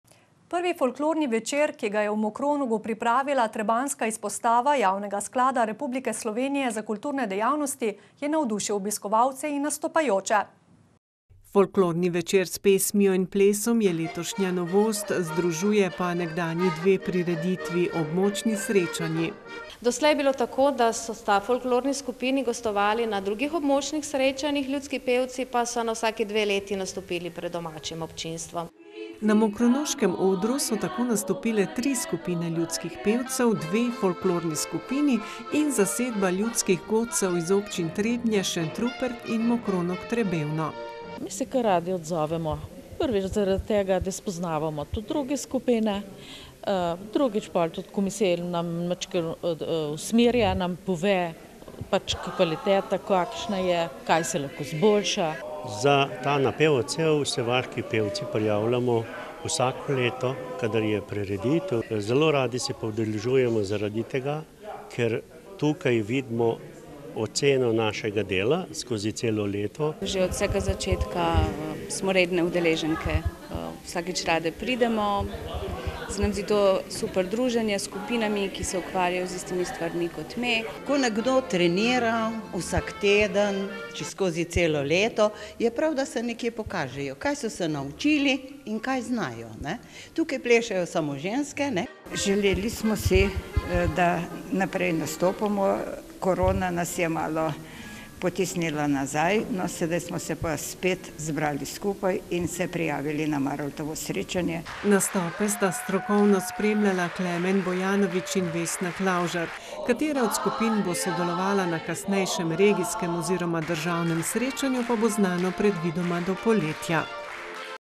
Folklorni večer s pesmijo in plesom
MOKRONOG - Prvi folklorni večer, ki ga je v Mokronogu pripravila trebanjska izpostava Javnega sklada Republike Slovenije za kulturne dejavnosti, je navdušil obiskovalce in nastopajoče.